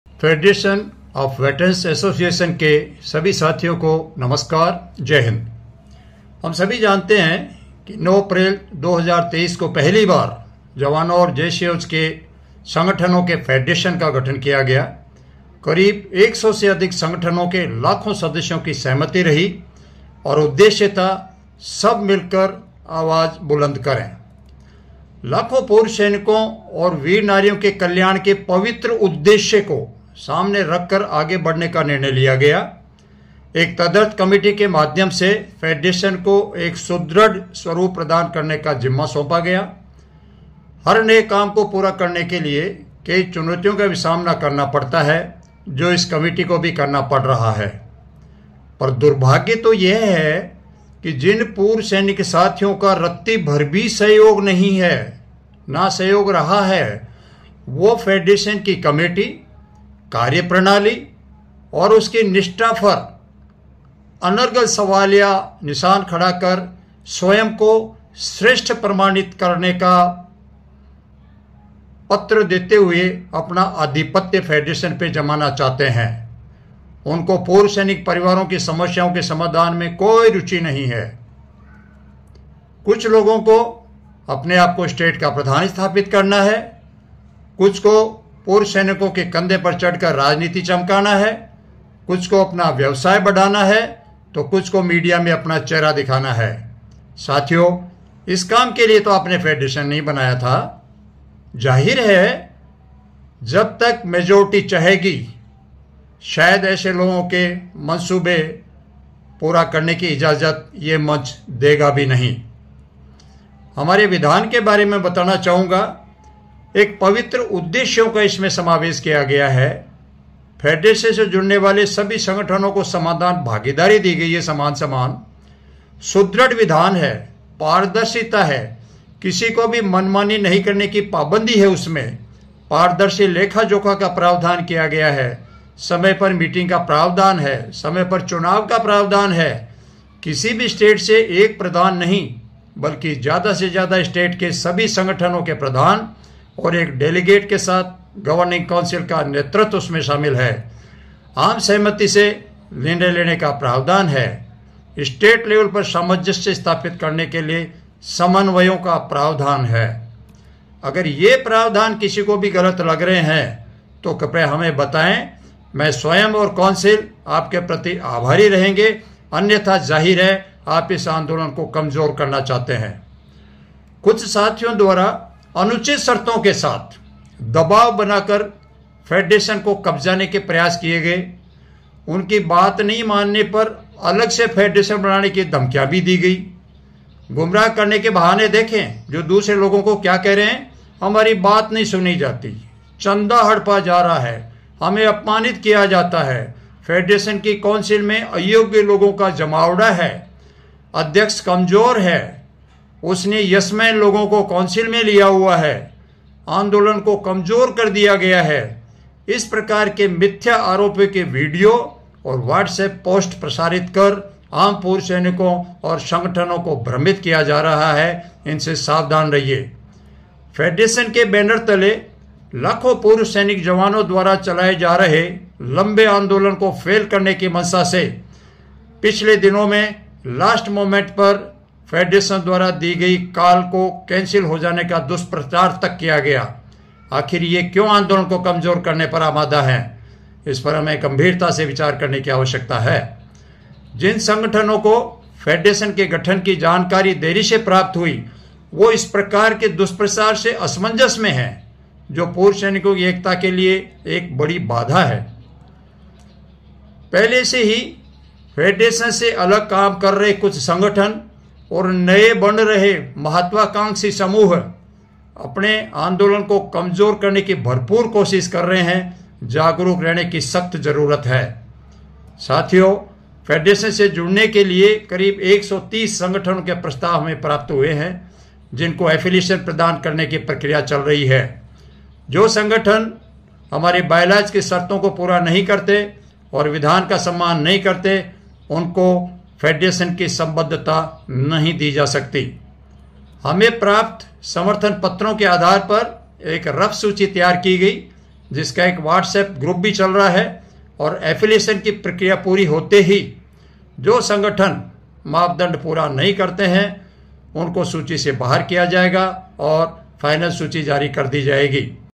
कुछ लोग निजी फ़ायदे के लिए FVA पे कब्ज़ा करना चाहते हैं 🤔fojifunda
EX- SERVICEMEN (JCOS/OR) SANGHARSH, OROP-2,

WE DEMANDS SETTLEMENT OF ANOMALIES, MAKE EQUAL MSP FOR ALL, INCLUSION OF OROP FOR DISPARITY IN DISABILITY PENSION

हम विसंगतियों के समाधान की मांग करते हैं, सभी के लिए समान एमएसपी करें, विकलांग पेंशन में समानता के लिए ओआरओपी में शामिल करें

(1). OROP: Sir, as the Central Government hasissued a press release dated 23 December 2022,information has been given about theimplementation of One Rank One Pension. Inwhich it has been told that only the soldiers whohave done color service and the soldiers who havecome for pre-mature retirement before 01 July2014 will be given the benefit of one rank onepension and the soldiers who have come onpre-mature retirement after 01 July 2014 will notbe given the benefit of one rank one pension.Sir, jawans never get premature retirementbecause their agreement is of 15 + 2 years, whileofficers agreement is up to 58 years and if officerstake retirement before 58 years, then it is calledpremature retirement Sir, our demand is that all thejawans should be given the benefit of one rank onepension.5, army officers come on pension at the age of 58to 60 years, because of that officers should not getthe benefit of one rank one pension, as thedefinition of OROP is that "soldiers should be senton pension at the age of 32 to 40 years.